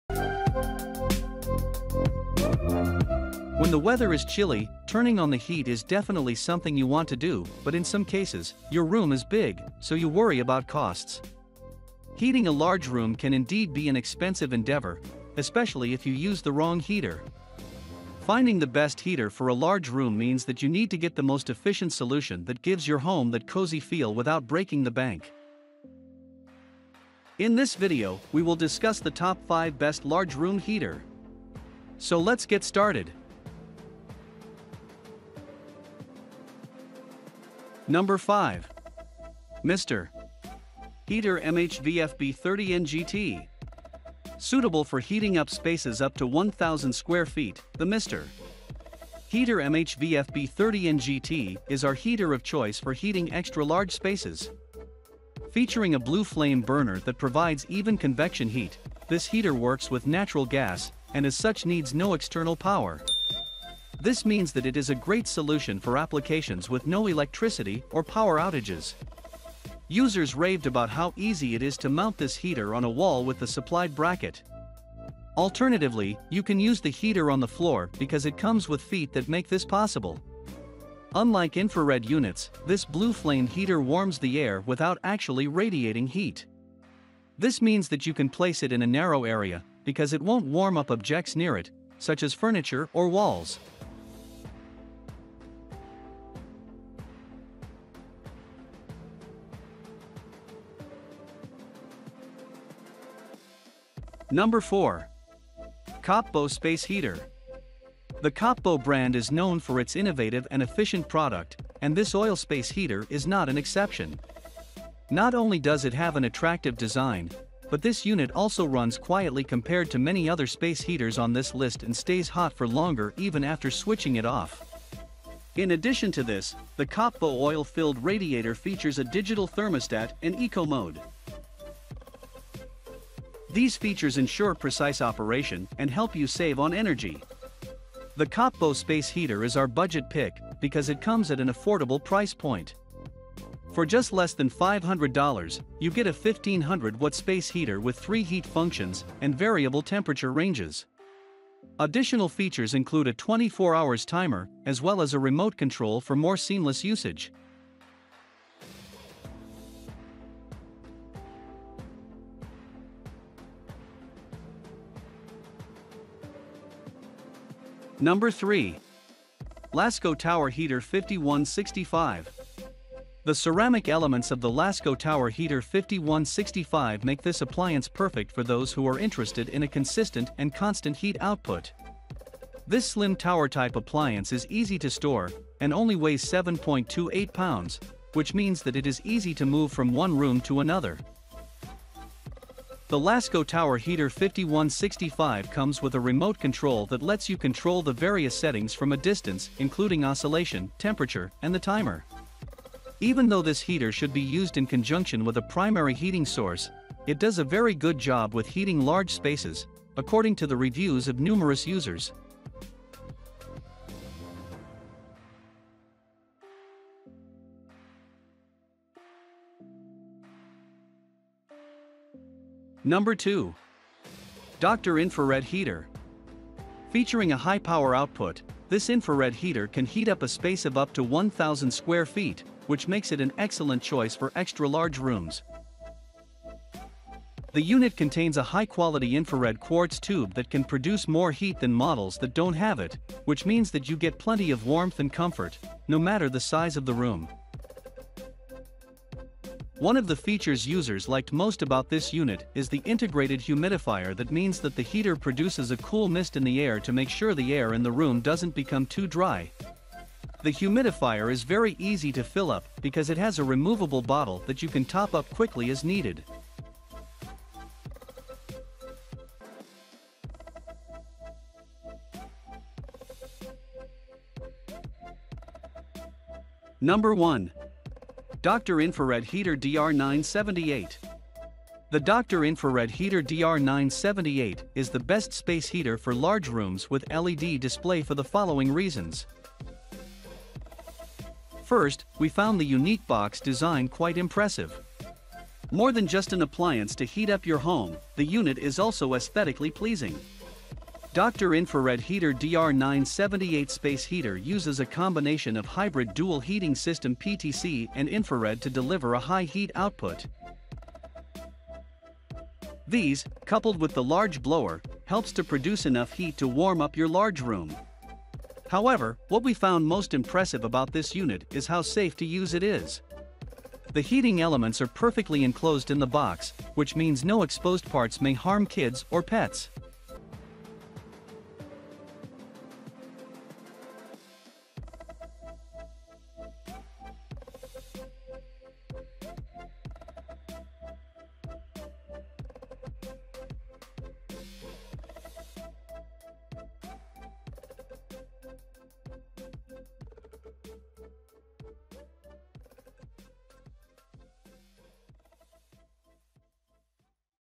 5 Best Large Room Heater [Reviews in 2024]
Best Large Room Heater:

00:00 - Intro

0:40 - 5. Mr. Heater 30,000 BTU  Natural Gas Heater

1:45 - 4. KopBeau Space Heater

2:50 - 3. Lasko 5165

3:44 - 2. Dr Infrared Heater

4:38 - 1. Dr Infrared Heater DR-978